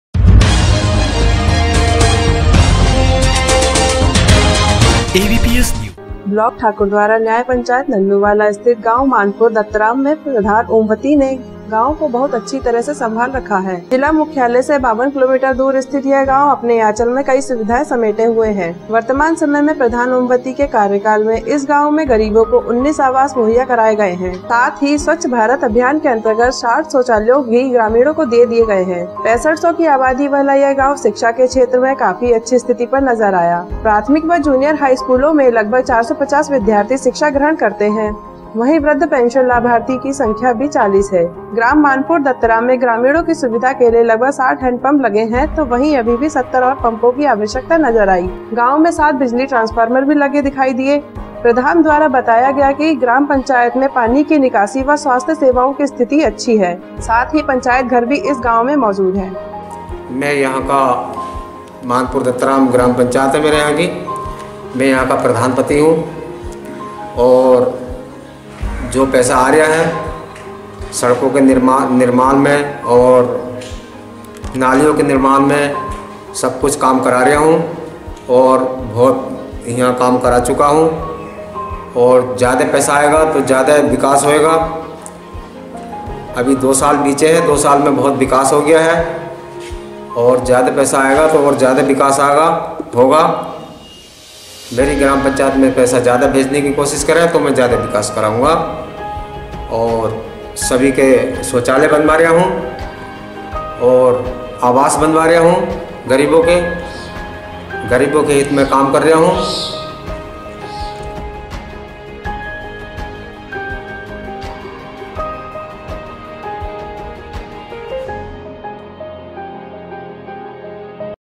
मानपुर दत्त राम गांव में प्रधान ओमवती ने किया है विकास कार्य
मानपुर दत्त राम गांव में प्रधान ओमवती द्वारा विकास कार्य लगातार किया जा रहा है साथी प्रधान पति ने सरकार से ज्यादा से ज्यादा पैसा भेजने की अपील भी की जिससे वह अपने गांव का अधिक से अधिक विकास करा सके